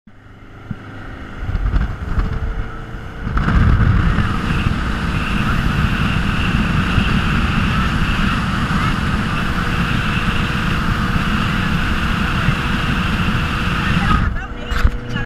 1989 Maxim Stingray 222 SS 454 Magnum Brovo 1 Hiting 60 miles per hour
Cranked "Sweetie"  Maxum 222 SS Stingray up to 60 MPH on a 4th of July ride.